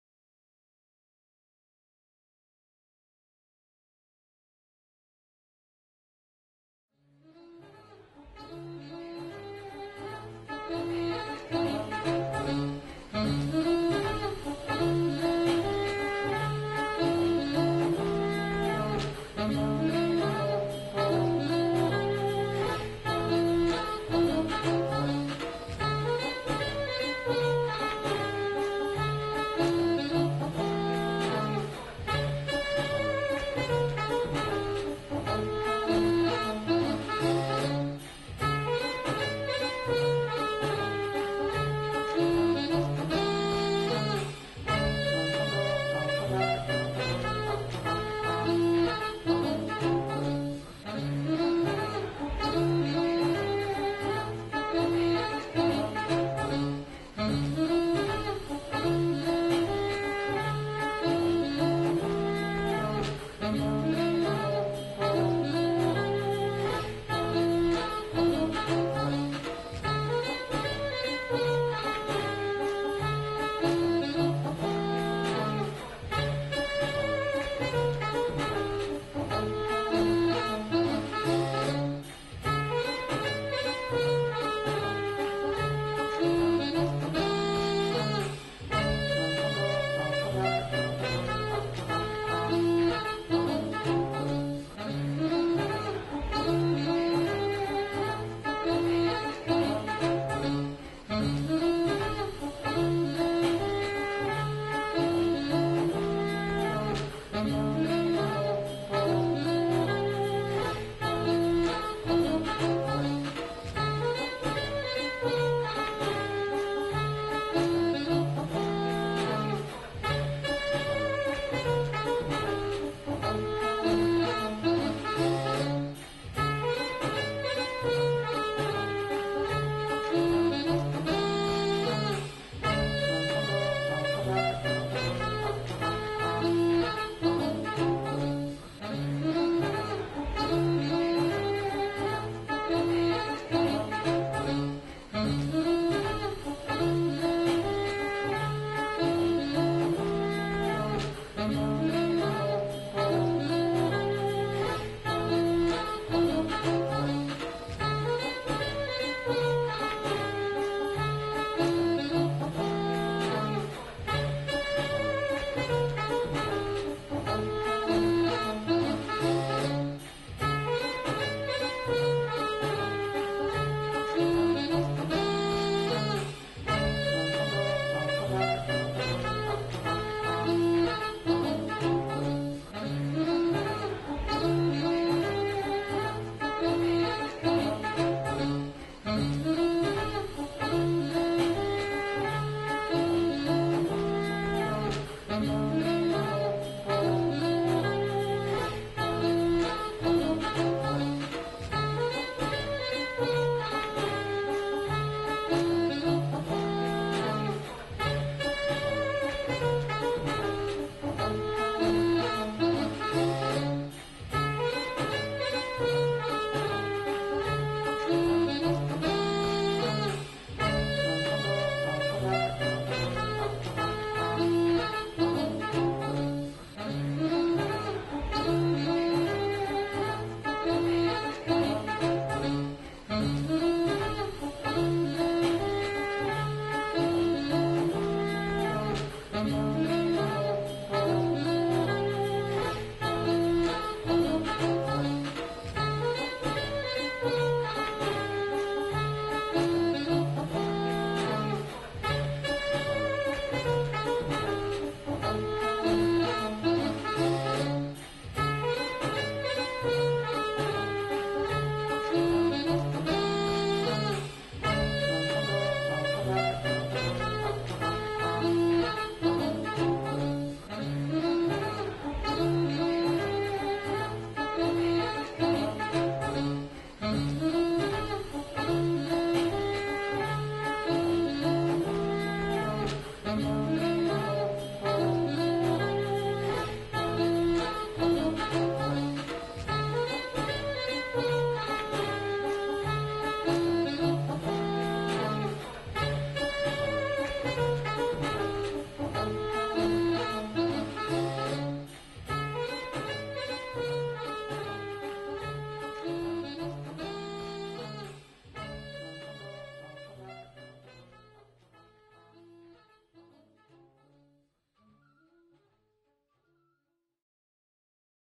The Comedy of Errors trailer
Highlights from the Australian Shakespeare Company's July 2010 production of The Comedy of Errors at the Athenaeum Theatre in Melbourne.